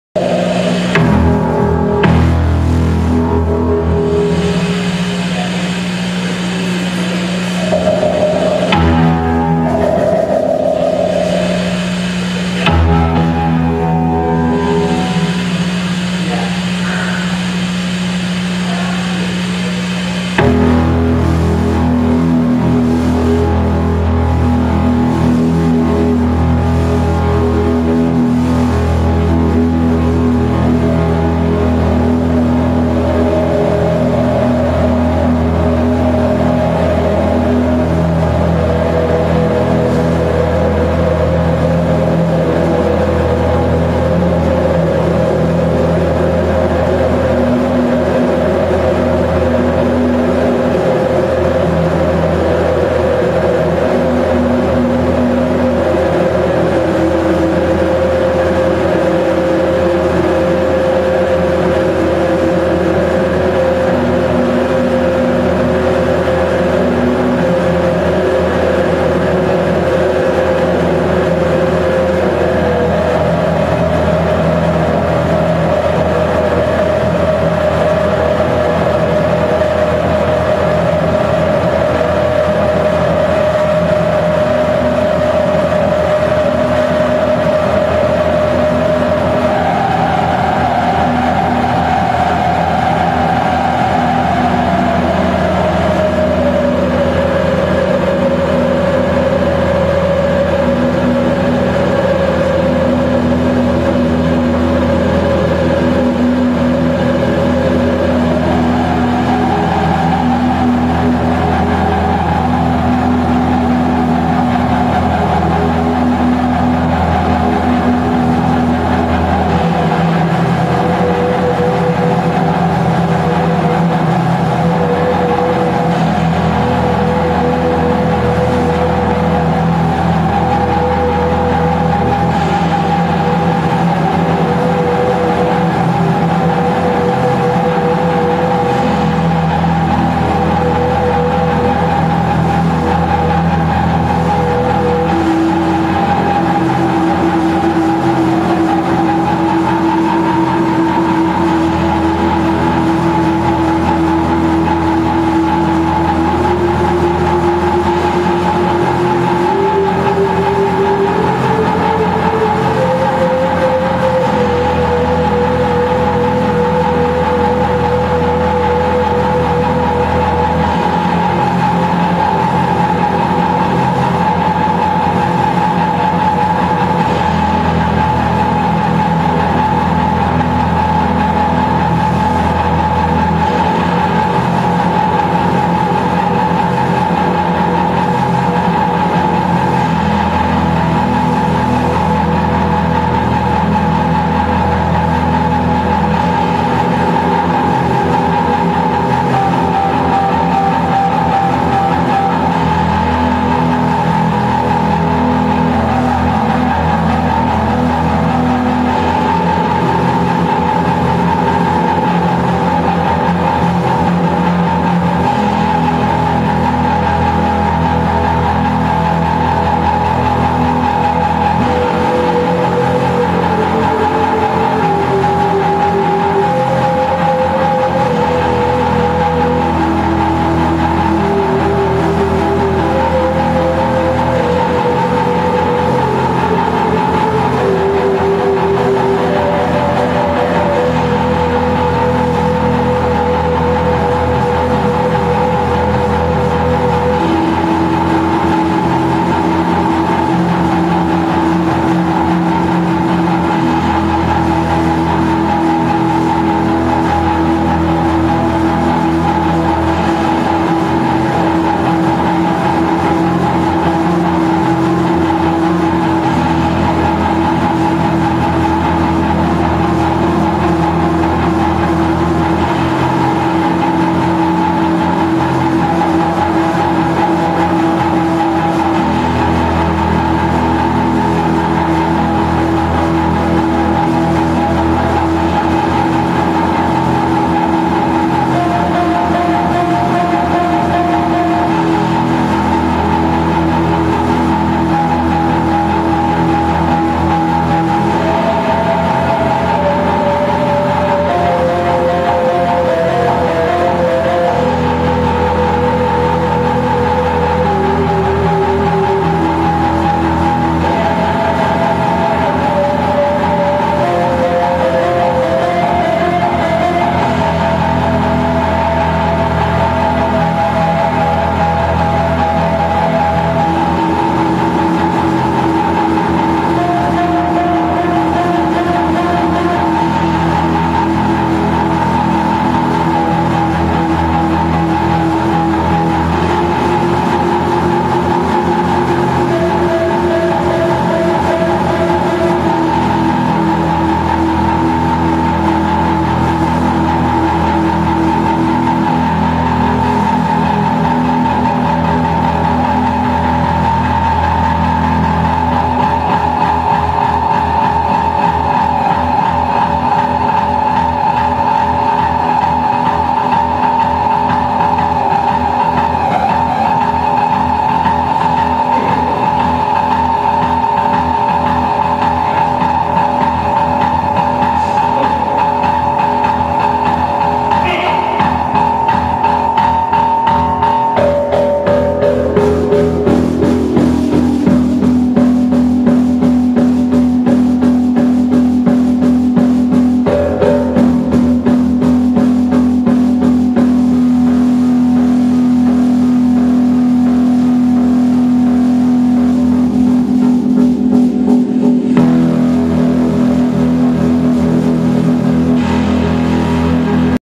Pinkish Black at the Raided X+ Art Opening
Pinkish Black recorded live on March 28, 2010 at the Kessler Theater in X+/North Oak Cliff (Dallas, Texas)